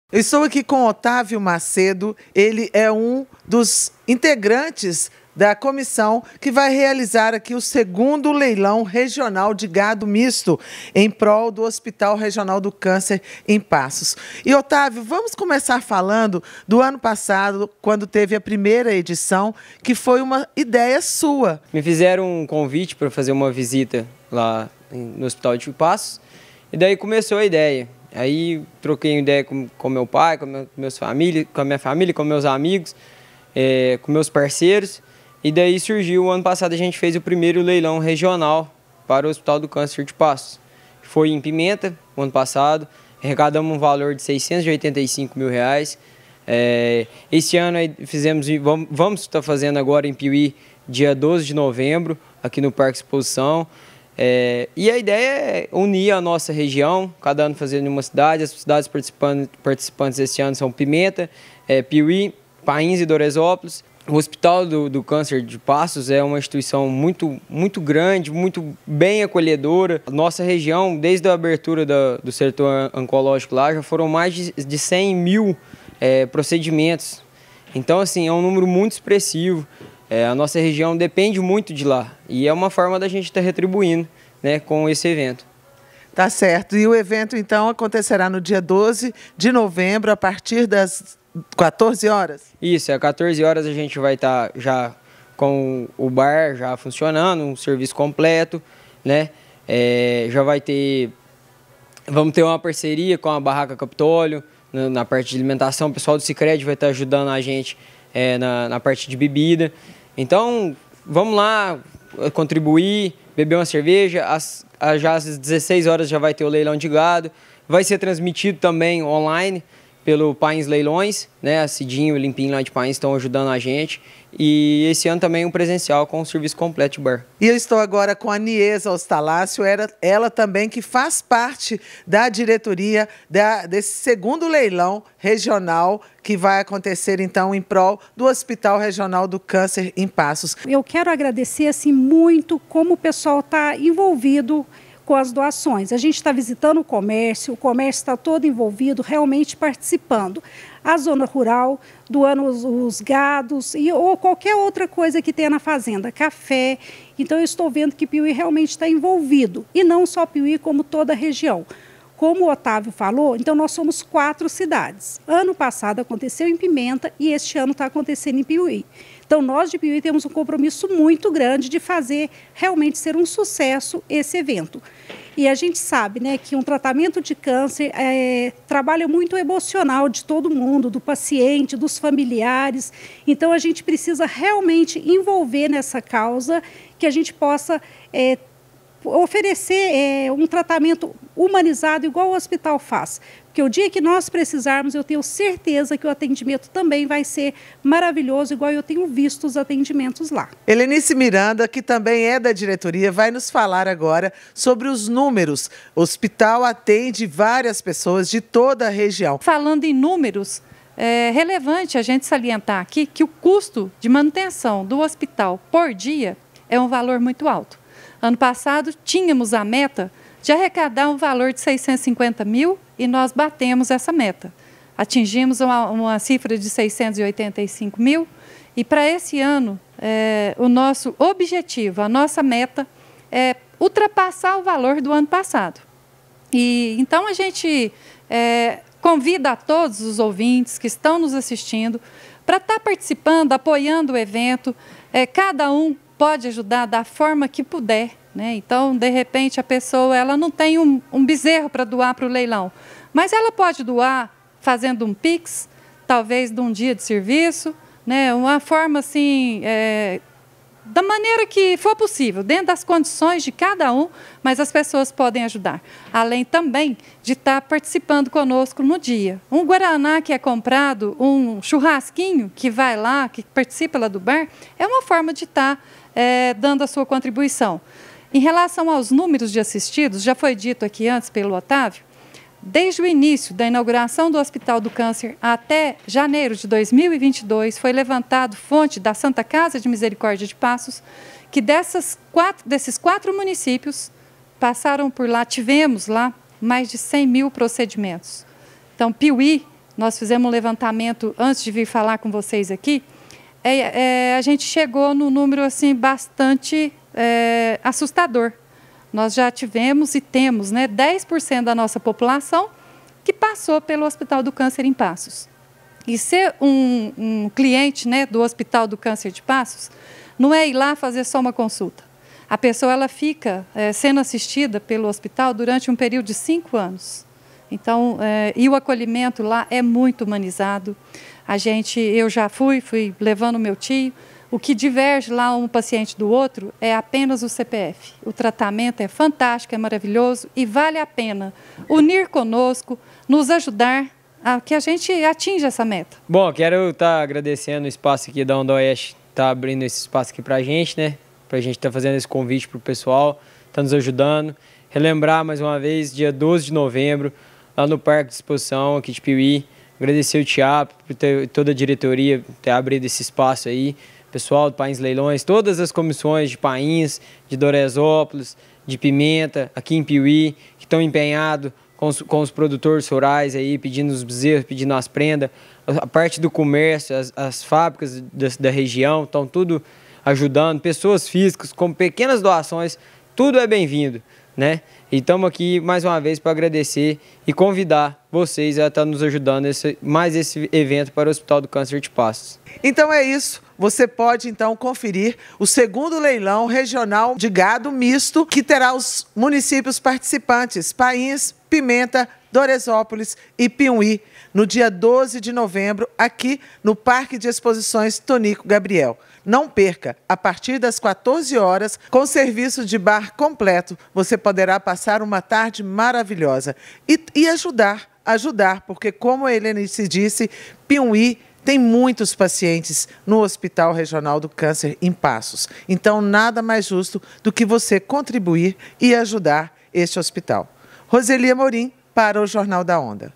HOSPITAL REGIONAL DO CÂNCER DE PASSOS JÁ ATENDEU MAIS DE 100 MIL PESSOAS DE PIUMHI E REGIÃO
| REGIÃO |
HOSPITAL REGIONAL DO CÂNCER DE PASSOS JÁ ATENDEU MAIS DE 100 MIL PESSOAS DE PIUMHI E REGIÃO. POR ISSO, VEM AI O 2º LEILÃO REGIONAL DE GADO EM PROL DO HOSPITAL. SAIBA COMO AJUDAR E PARTICIPAR: